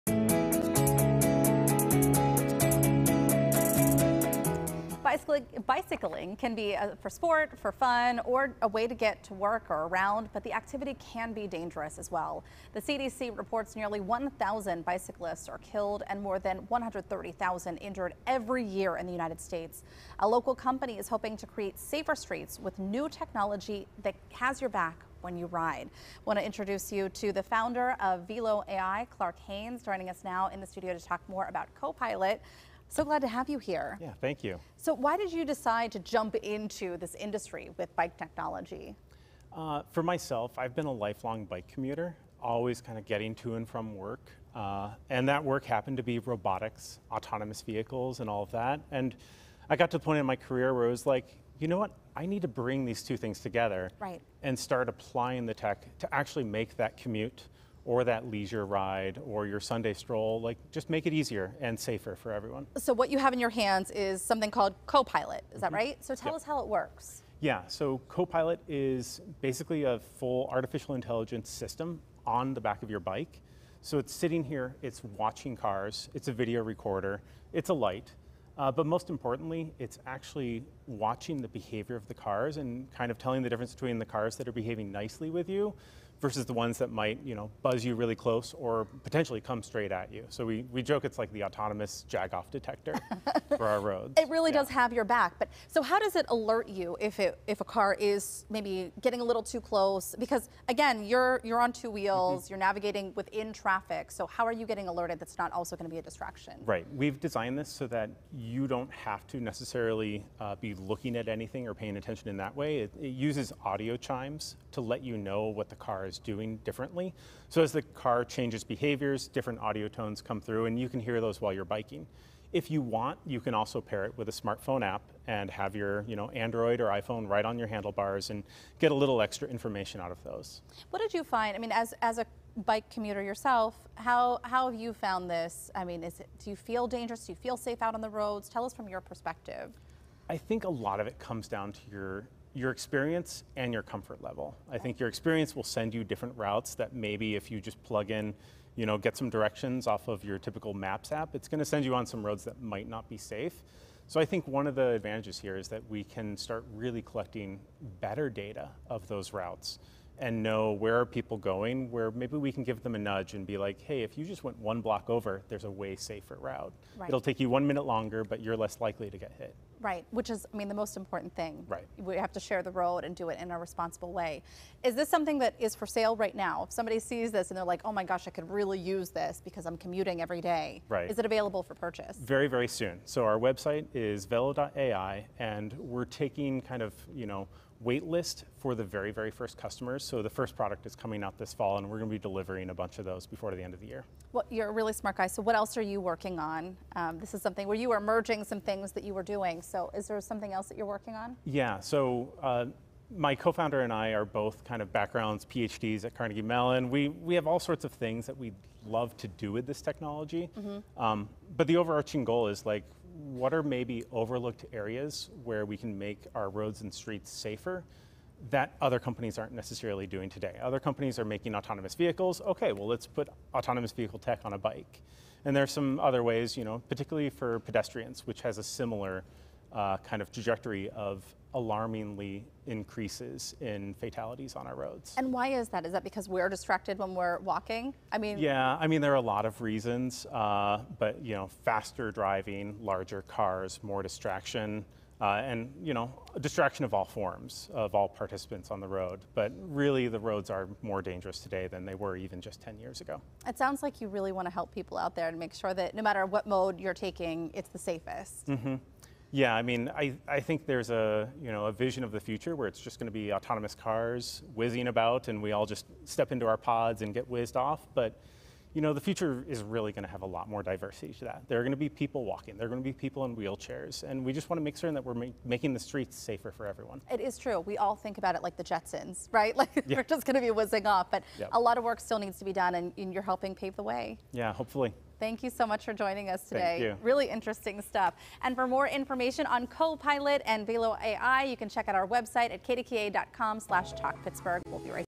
New technology that can protect bicyclists on the street
The CDC reports nearly 1,000 bicyclists are killed and more than 130,000 injured every year in the United States. A local company is hoping to create safer streets with new technology that has your back when you ride. Clark Haynes, the founder of Velo AI, joined us in-studio.